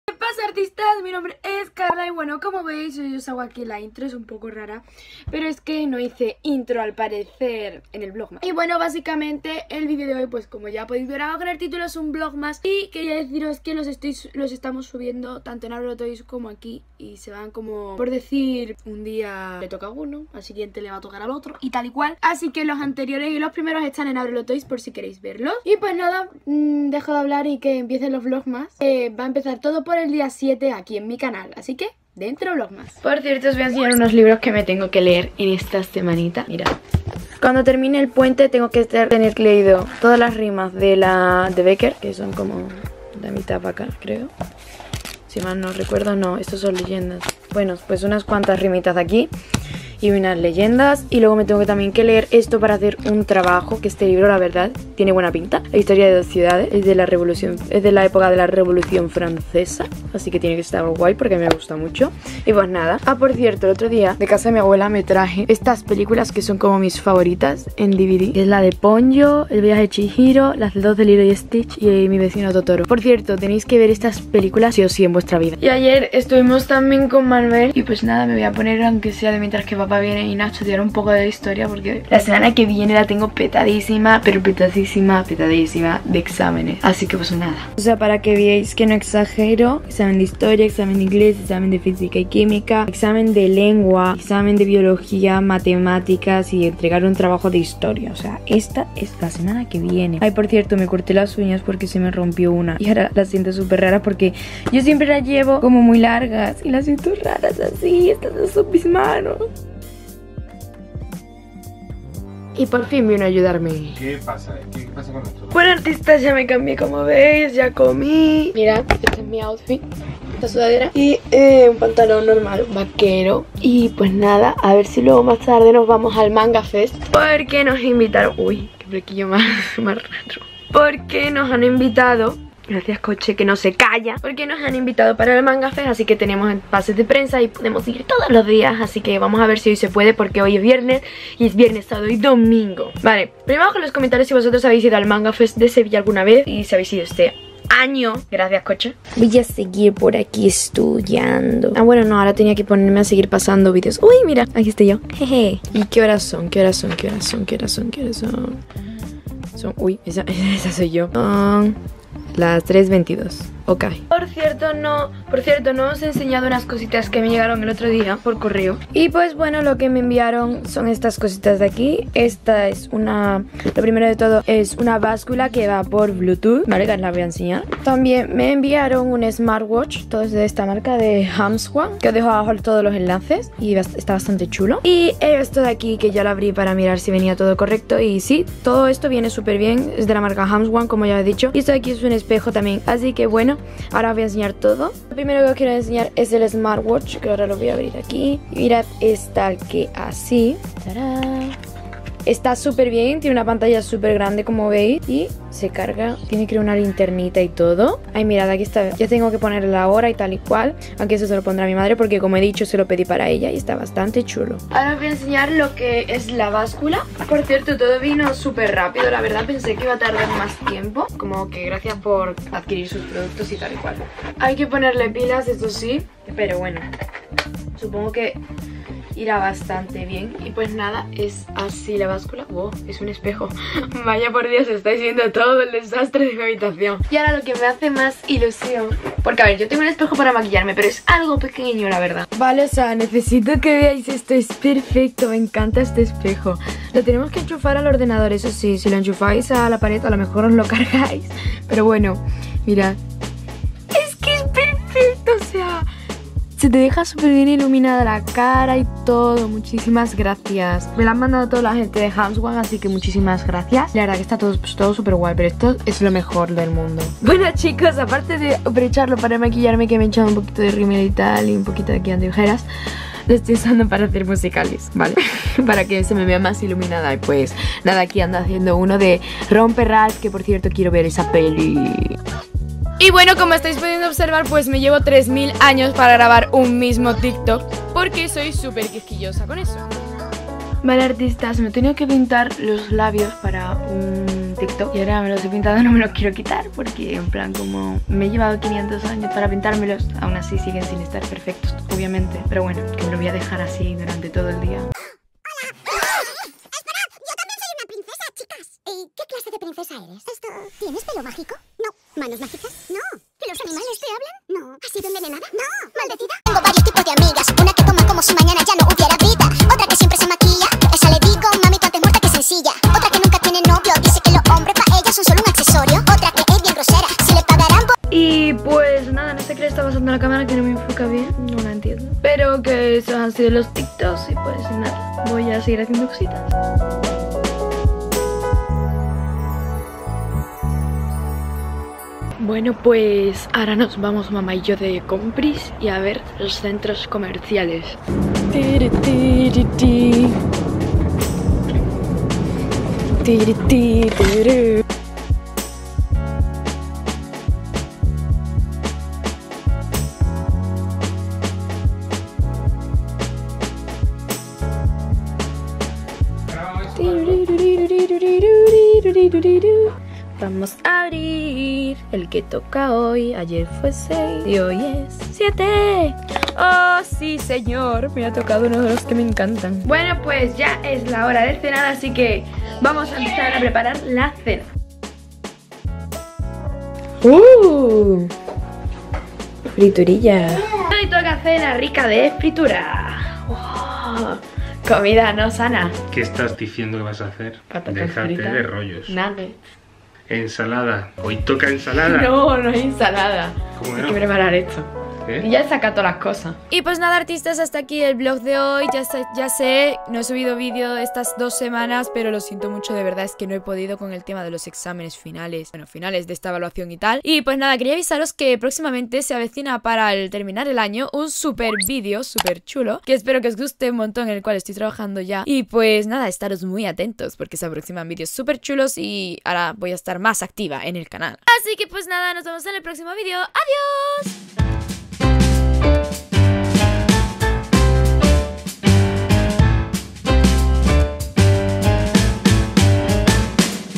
VLOGMAS día 7 | Haciendo TIKTOKS ( musical.lys )
Hoy os traigo un VlogMas del día 7 en el que estuve haciendo tiktoks o música.lys

AQUÌ PODRÉIS ENCONTRAR LOS PRODUCTOS DEL VIDEO

KarlaArt:
Apartado Correos 14
41900 Camas
Sevilla - España

twiter : @artistika26